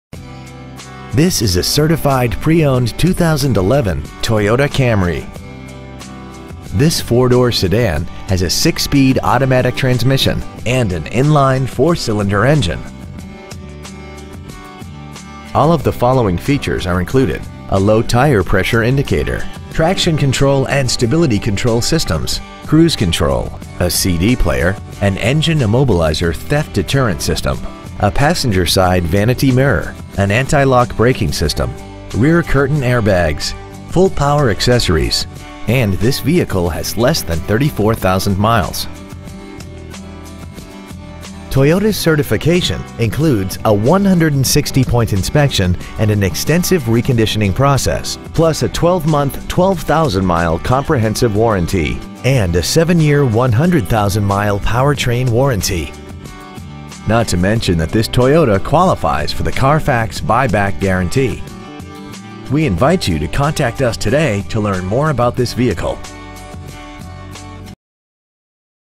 2011 Toyota Camry Certified San Antonio Texas 78238
Year : 2011
 Make : Toyota
 Model : Camry
 Engine : Gas I4 2.5L/152
 Trans . : Automatic
 Exterior : Classic Silver Metallic
 Miles : 33,122
 Interior : Black
 Stock : 21899B

 5730 N.W. Loop 410

 2011 Toyota Camry San Antonio Texas